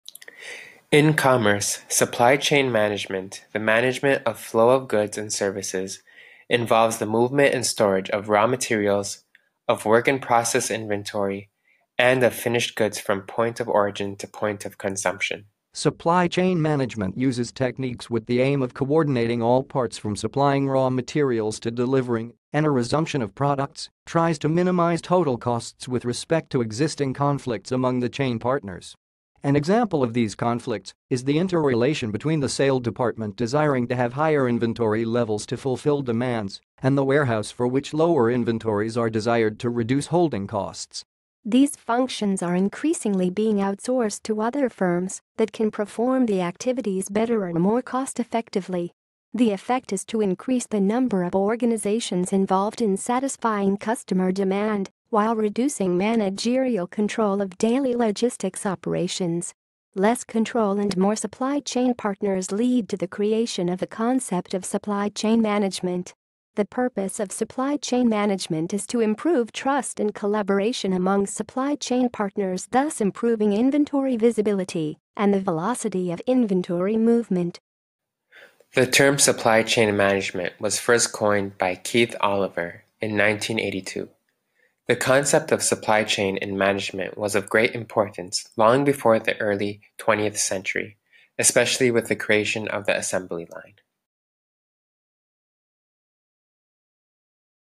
See how your mundane PowerPoint Presentation can be enhanced with Squigl.
You can leave a slide as is, convert any or all slides to animated images, record your own voice, choose machine voice-over or use someone else's voice for the entire presentation or just one slide. In this example, several voice-over choices were used to illustrate this fact.